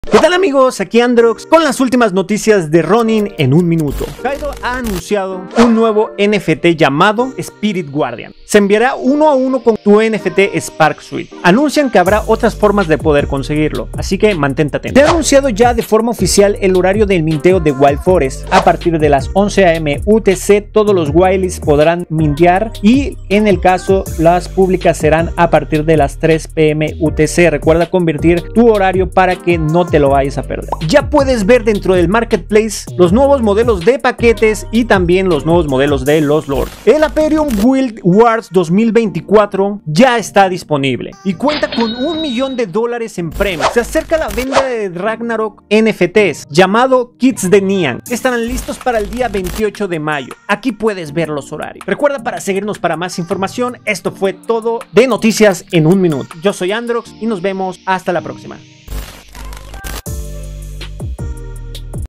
HORARIO DE MINTEO WILDFOREST NOTICIAS RONIN EN 1 MIN
¡Bienvenidos a Noticias en 1 Minuto de Ronin! 📰🚀 En el video de hoy, te traigo las últimas novedades sobre algunos de los juegos más emocionantes en el ecosistema Ronin:

🔥 Kaidro: Descubre las nuevas actualizaciones.
🌳 Wildforest: Horarios de Minteo!
🌌 Apeiron: Guild Wars!
⚔️ Ragnarok: Minteo dia 28 de Mayo!